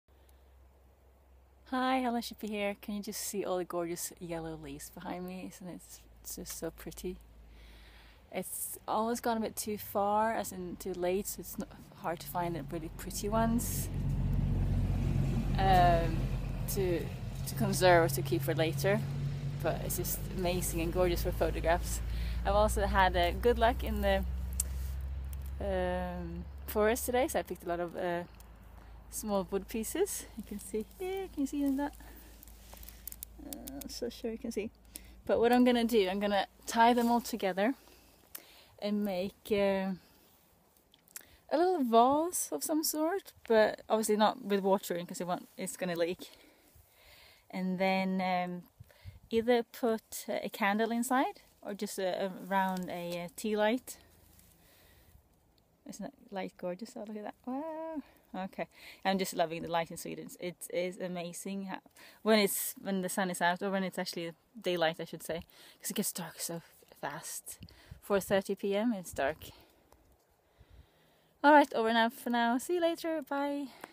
Handcrafts with forest twigs
Been out in the Swedish forest and picked twigs that i will bind together and make ornaments of.